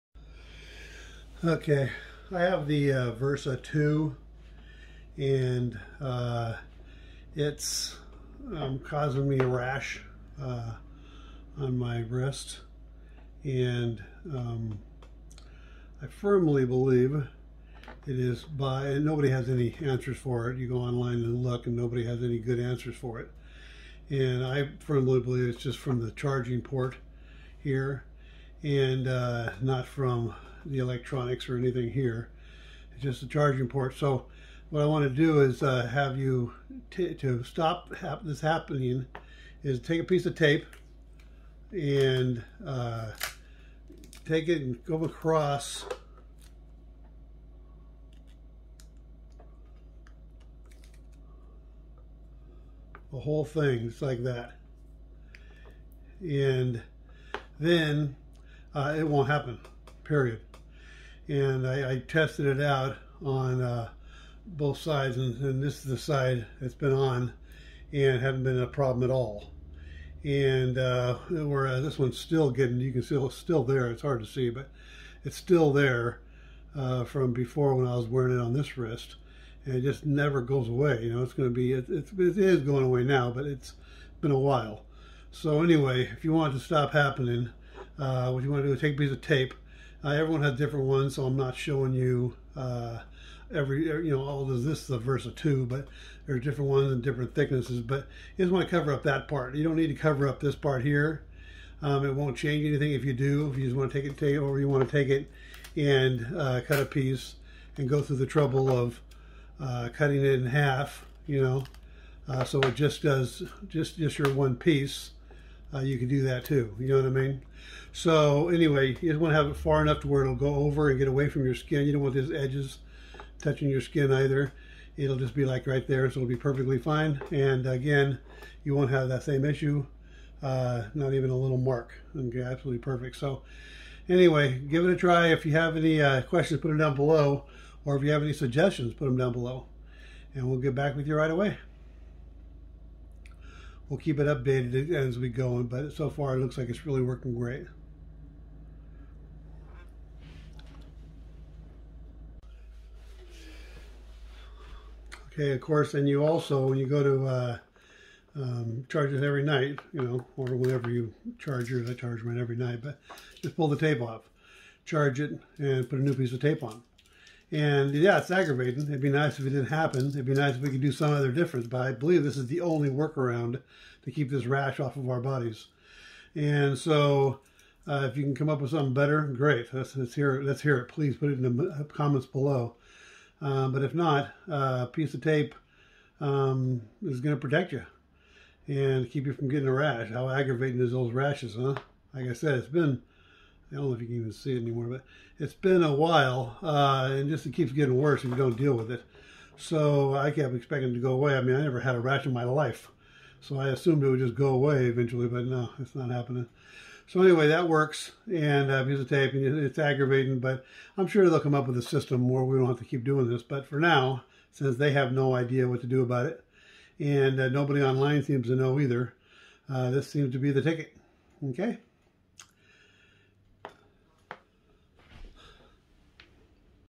Fitbit smartwatch rash/solved
I have the versa two, but it works with all the fitbits